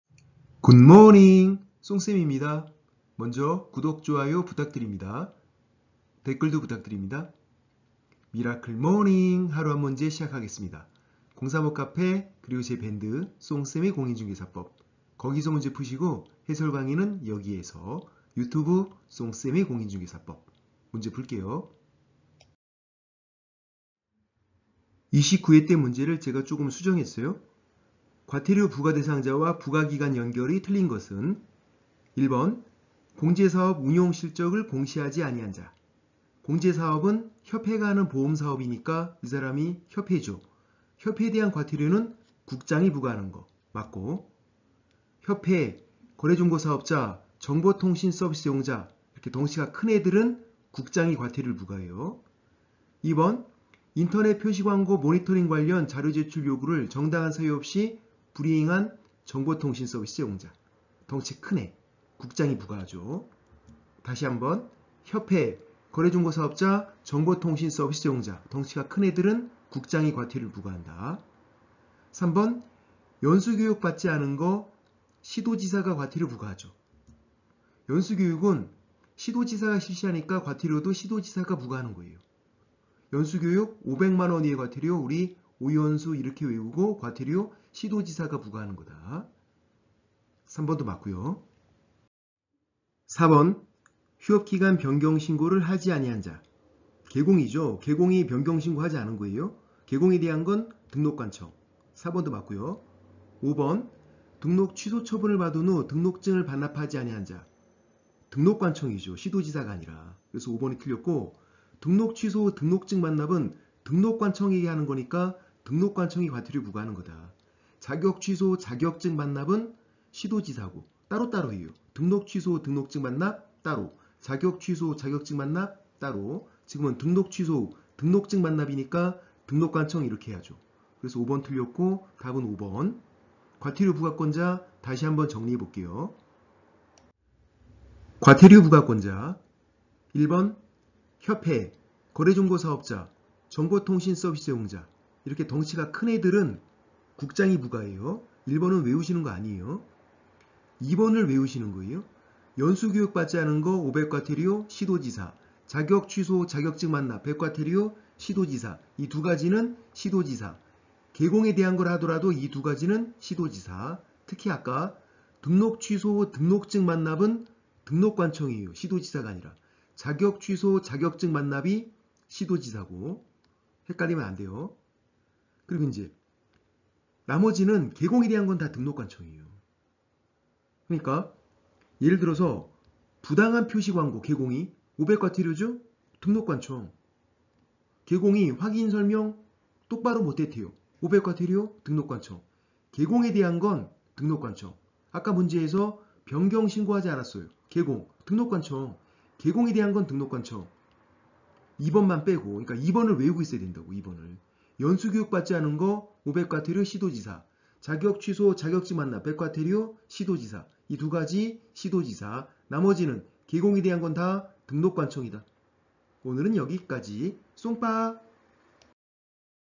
제35회 대비 미라클 모닝 하루 한 문제(7월 12일)
공인중개사법 하루 한 문제를 풀면서 아침을 시작합시다~! 하루 한 문제로 중개사법 고득점의 기적을 만듭시다~! 하루 한 문제지만 누적되면  시험보기 전까지 200문제를 풀게 되기에 실현 가능한 기적입니다.

월요일부터 금요일까지 업로드되며, 문제는 제 밴드 송쌤의 공인중개사법 또는 네이버 카페 공사모에서 푸시면 됩니다.